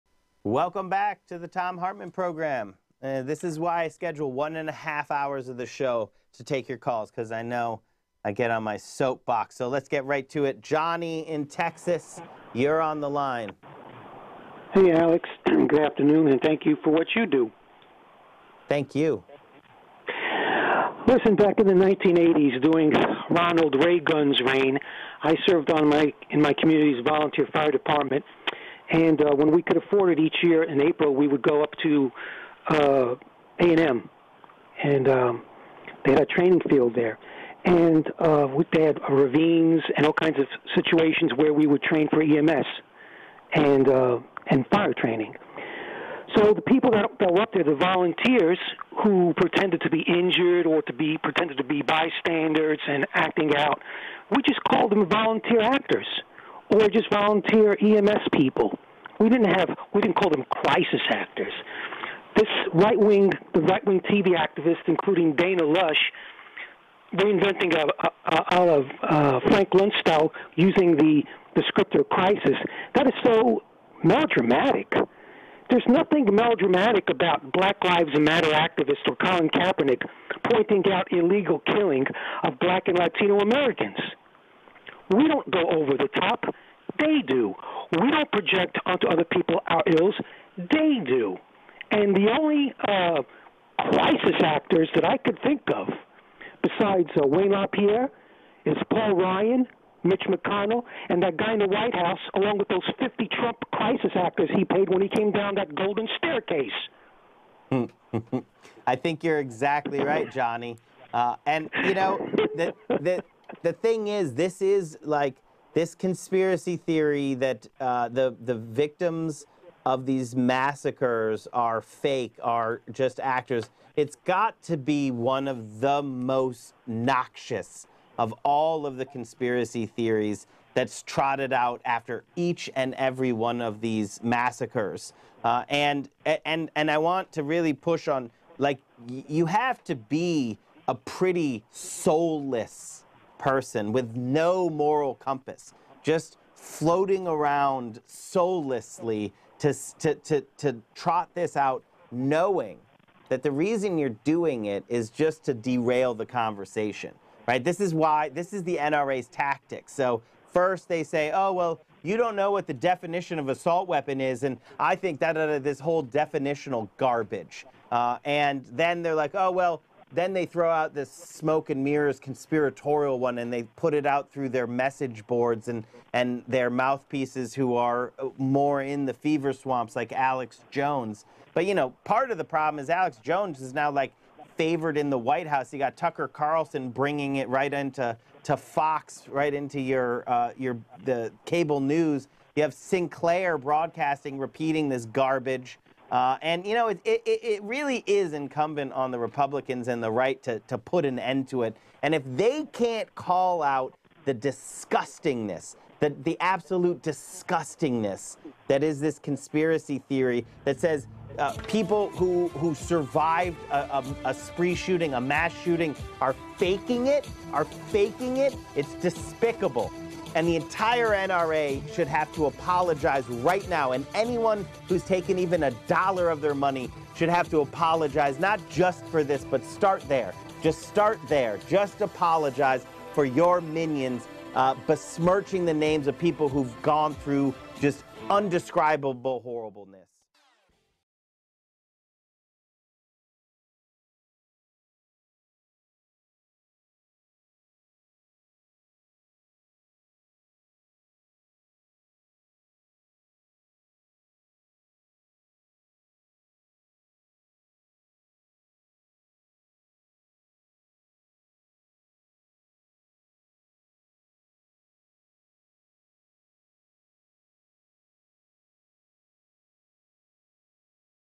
Is Wayne Lapierre Just a Crisis Actor Performing For Big Money?
The worst parts of the internet are fuming with conspiracy theories about the survivors of the tragedy at Parkland, calling them Crisis actors, that they are paid to protest but aren't Paul Ryan, and Wayne Lapierre paid to protect the interest of the billionaire, morbidly rich class?